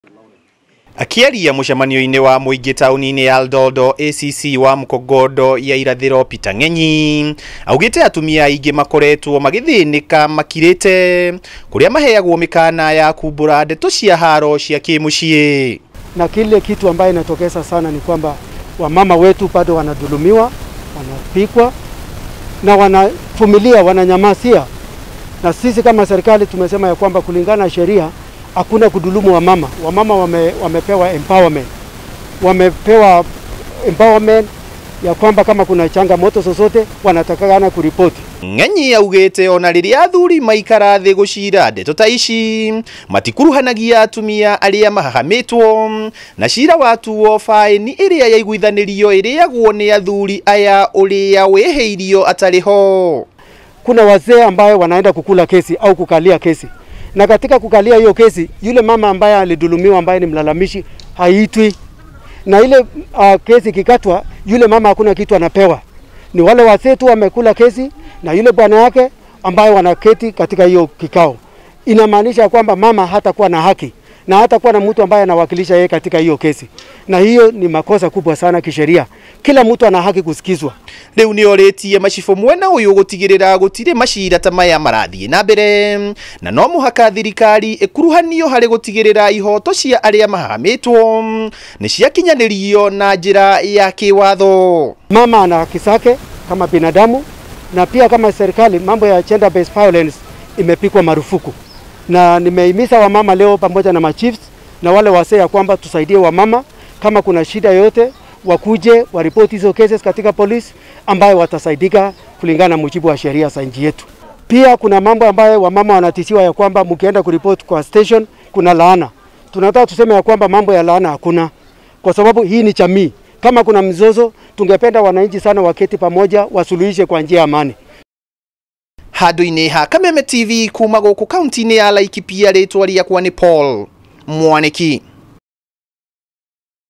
Thirikari guthii nambere na kambi-ini cia kunina ugimarithia wa airitu Laikipia
Atumia mwena wa Laikipia ya rugongo nimoritio kuhuraga riboti riria manyamario na haaro cia kimucii ni athuri ao kuona ciiko nyingi cia kunyamarania ni irathii nambere mwena uyu.
Uu ni thutha wa ndeto kumbuka ati atumia aingi makoretwo makigirika riria mahahamwo ni athuri ao mwena uyu kuria meragirwo ati mangihura riboti cia ndeto ici nomatigirwo irumi ni athuri.